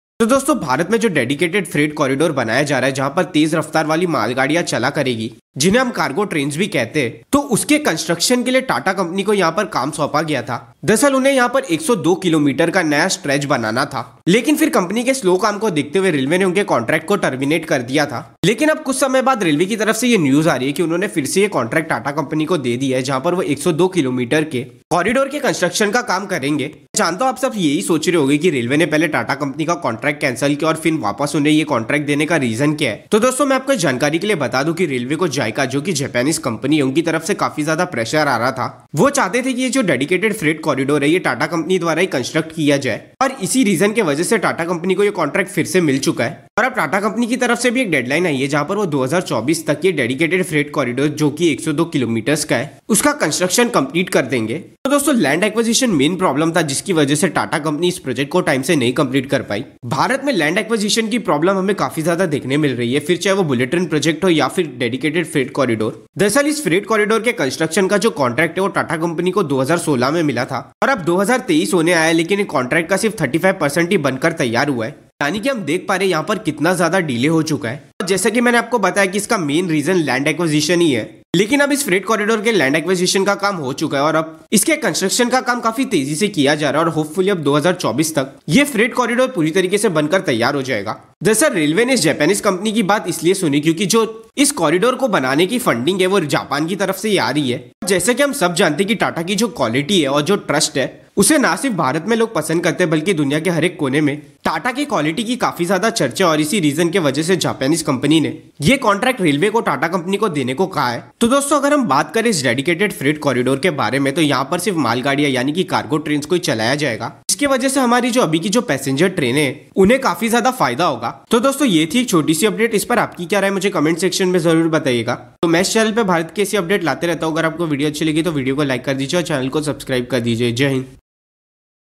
पूरे एशिया में पहली बार बना दिया दिखा डाली ना औकात
Asia first's dedicated freight corridor operational in india soon.